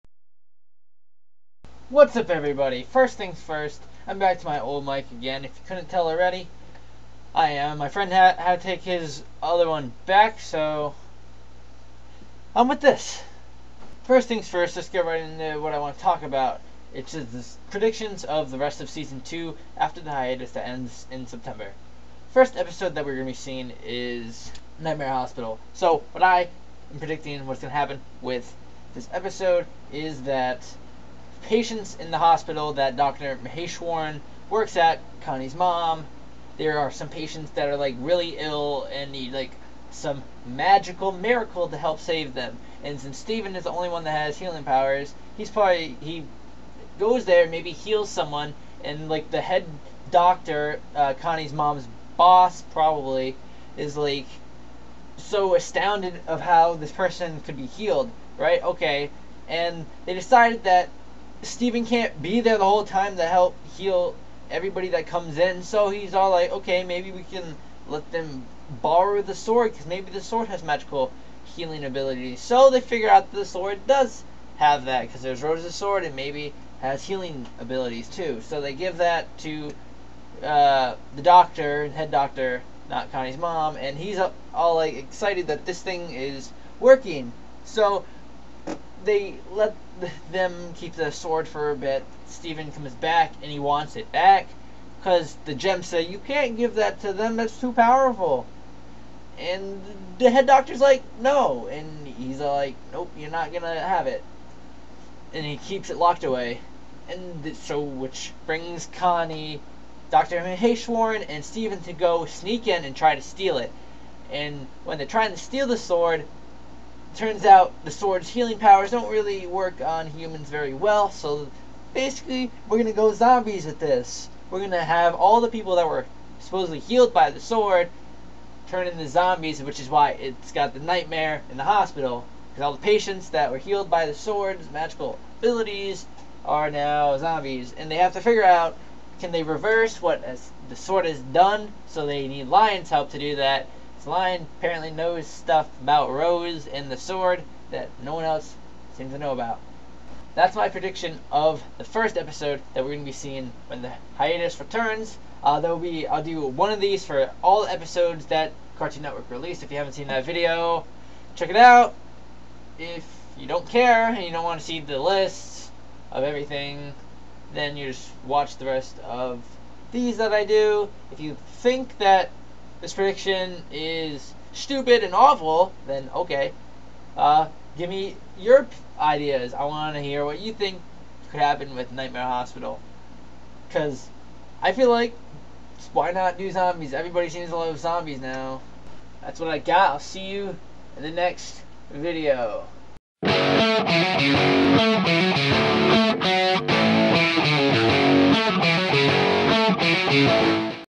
Steven Universe ~ Season 2: Nightmare Hospital (Predictions/ discussion)
This is my predictions before the airing of the episode nightmare hospital and what may happen. Tell me what you guys think could happen in this episode.